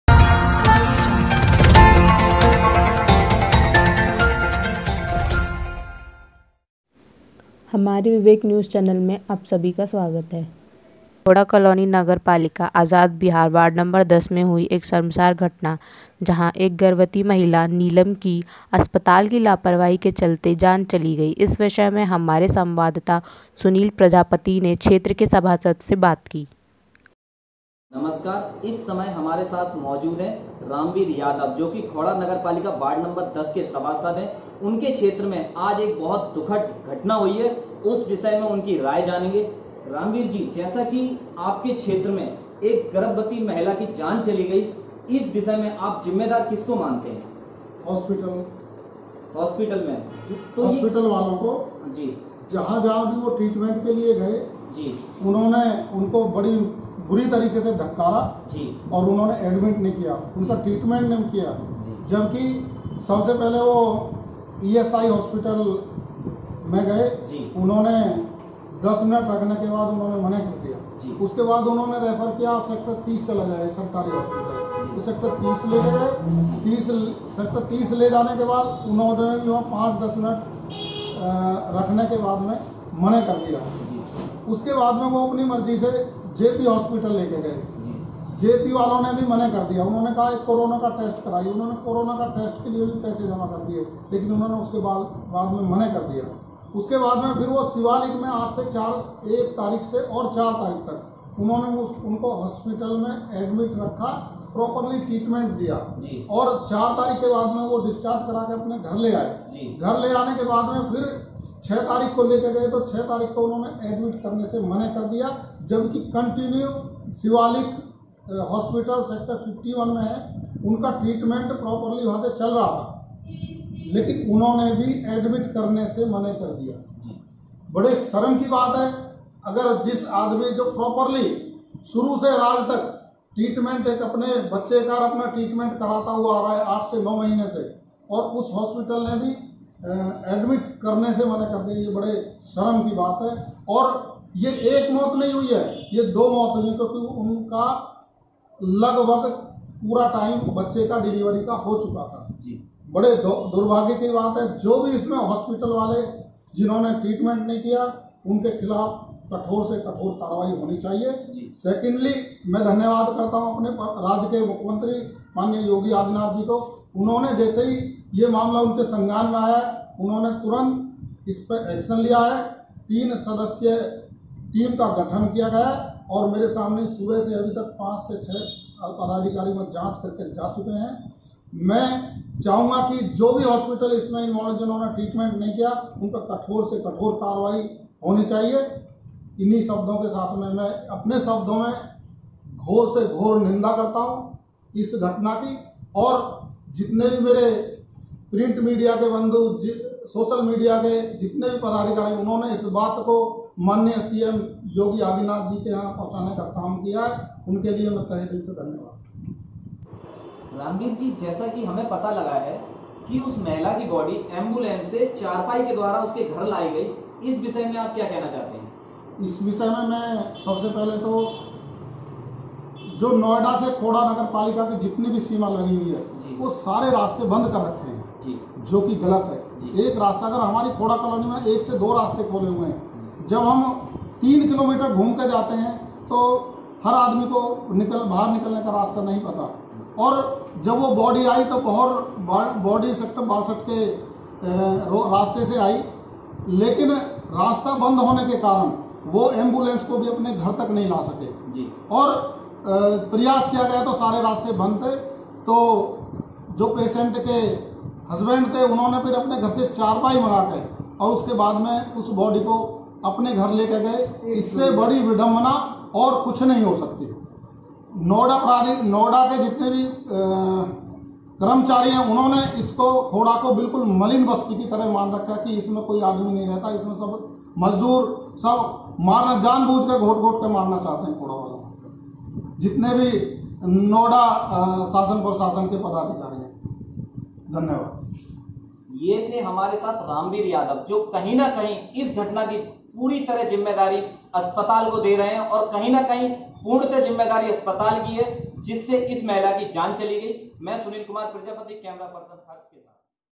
खोड़ा नगर पालिका के आजाद बिहार वार्ड नंबर 10 में हुई एक शर्मसार घटना जहां एक गर्भवती महिला नीलम की
खोड़ा कॉलोनी गाजियाबाद